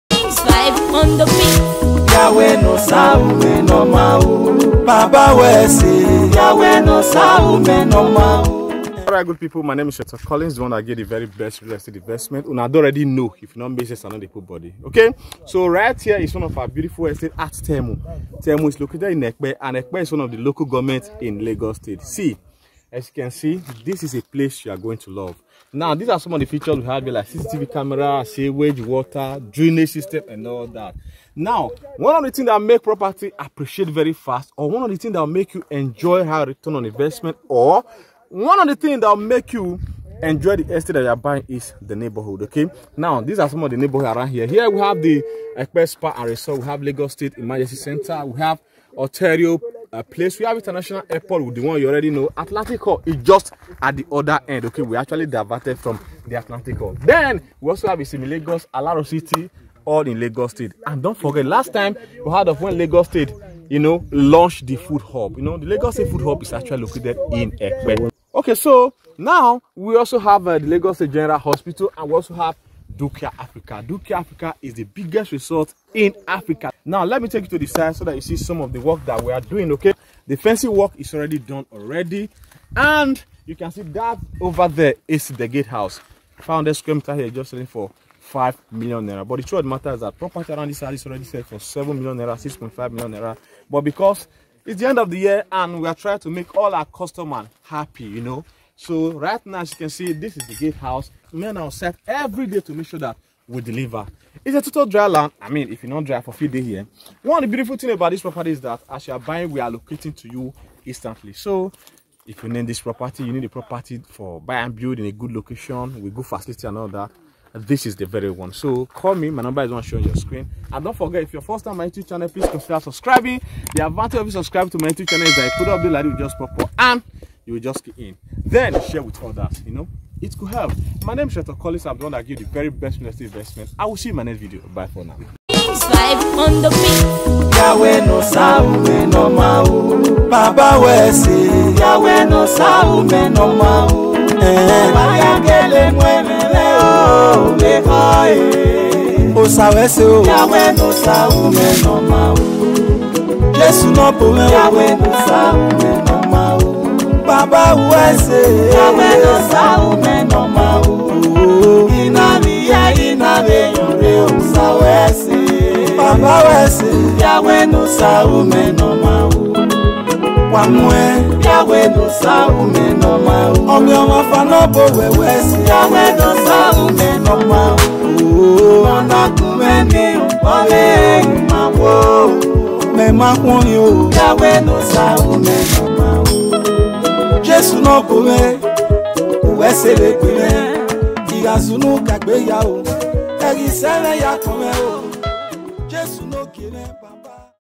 Affordable Dry land for sale in Epe near T-Junction - Isimi Lagos - Dukia Africa Elite Ville Estate
See Affordable Dry land for sale at Elite ville Estate Epe very close Epe T-Junction, Isimi Lagos and Dukia Africa.

also available on whatsapp

5M for 500sqm
Excluding development and documentation fees